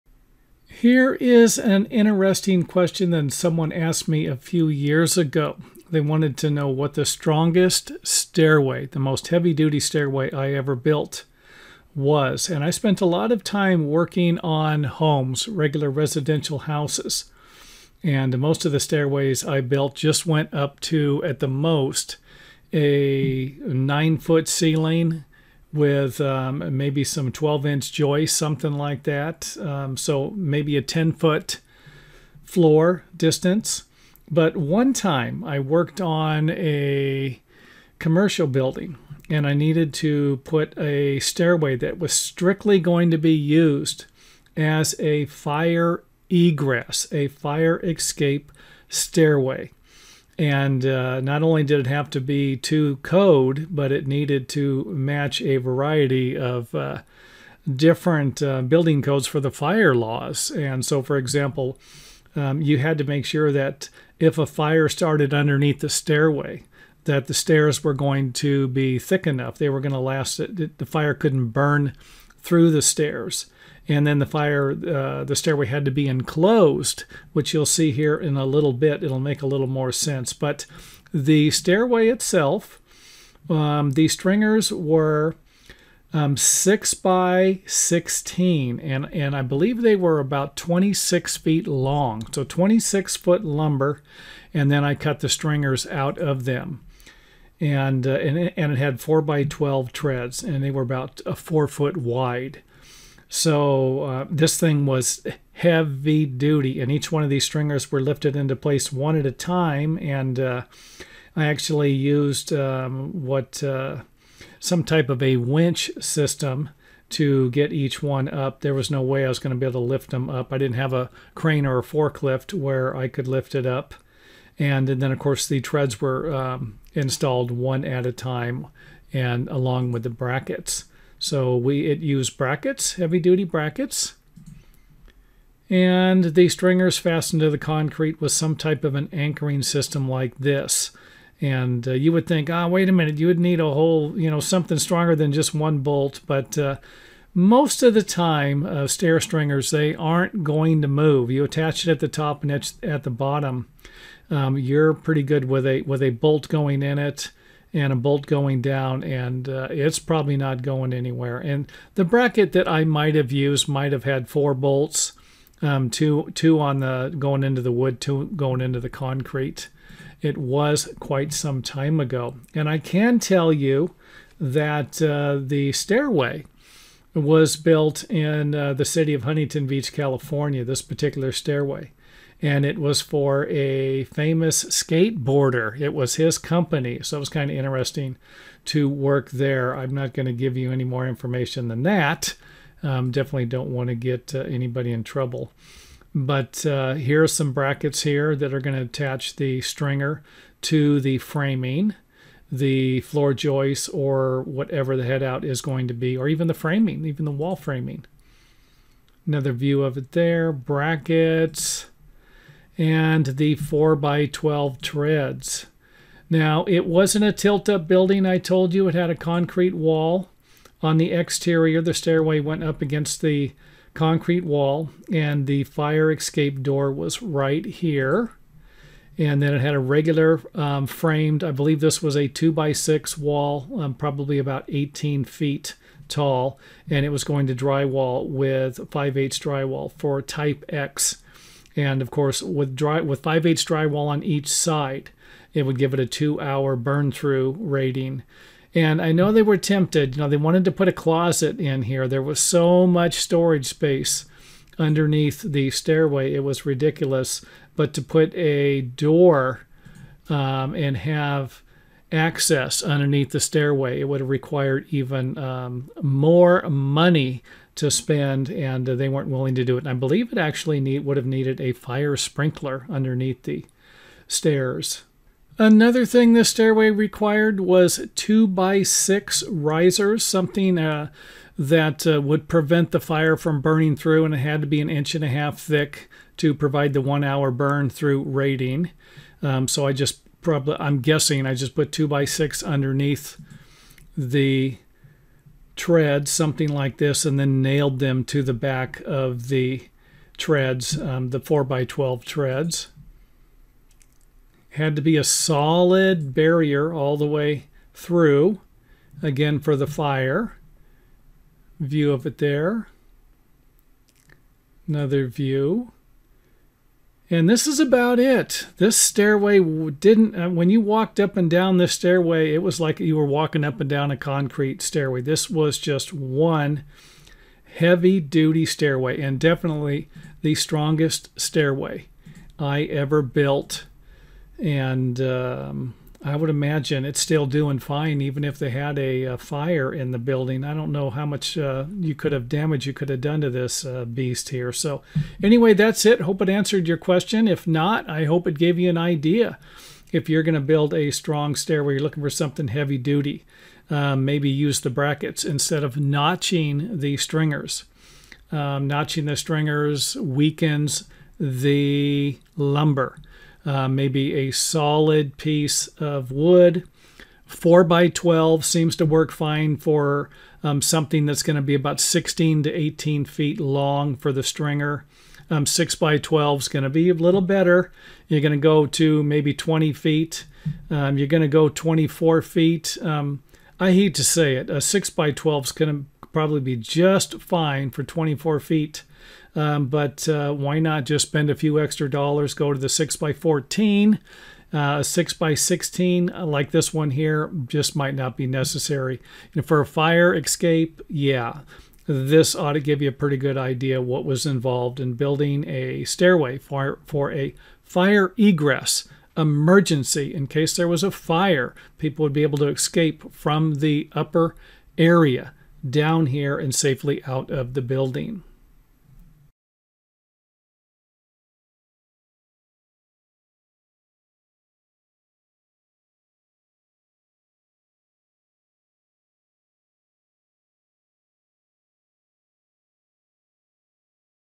What Is The Strongest Wood Stairway I Ever Built? - Structural Engineering And Fire Egress
Click on this link for more information about stair building, design and even repairs. This video will provide you with an excellent example of a stairway I built a few years ago that was without a doubt the strongest stairway I ever constructed and it was for a fire egress in a large building. The reason why make the video was to provide viewers with an example of a stairway that could be built for the same purpose.